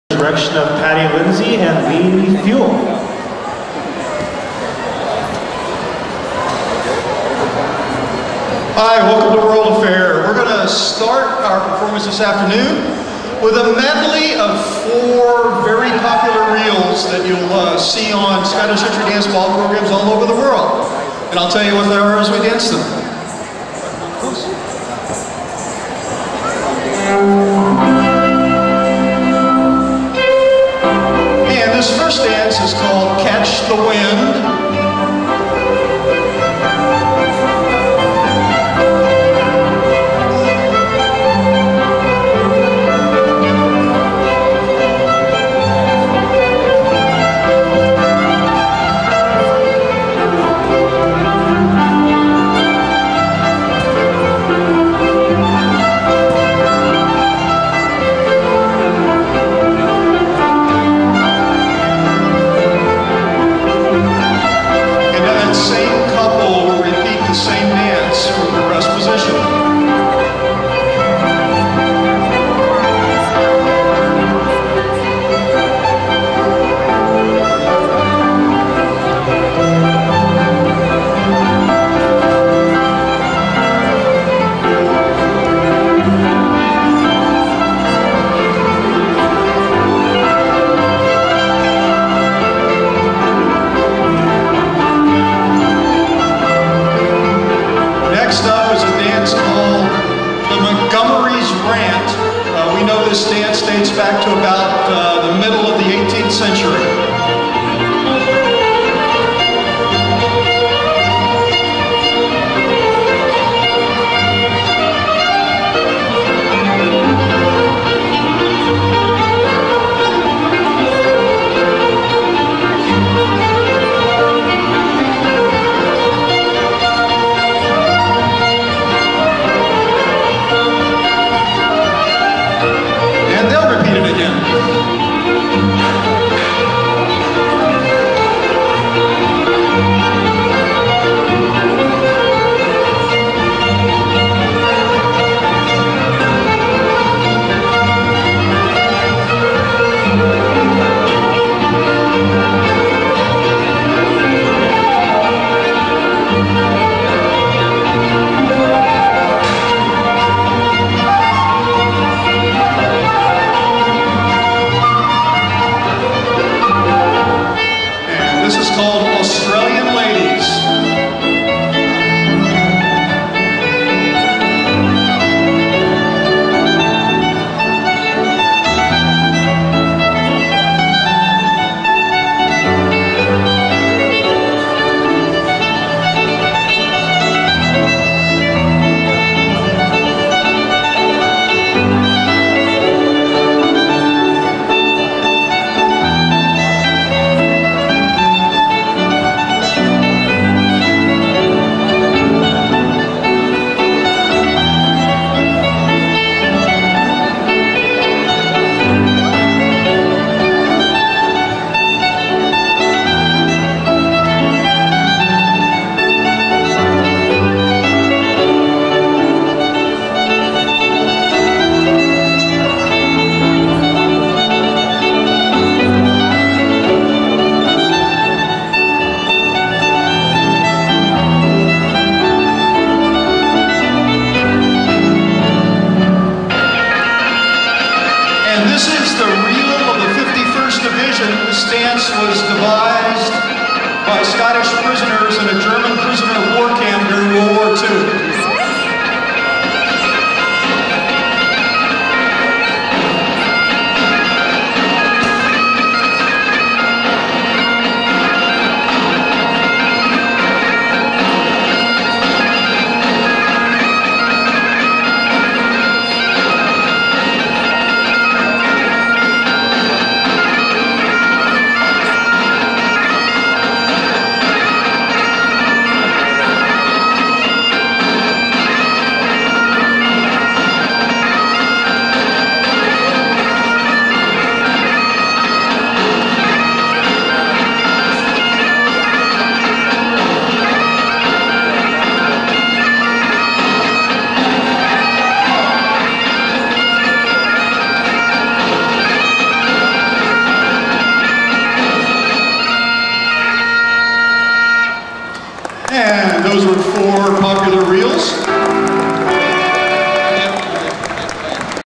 Flying Ghillies World A'Fair Saturday 18 May 2013 -- Popular Reel Medley
A collection of four dances in a medley at Dayton's World A'Fair:  Catch the Wind, Montgomery's Rant, Australian Ladies, and Reel of the 51st Division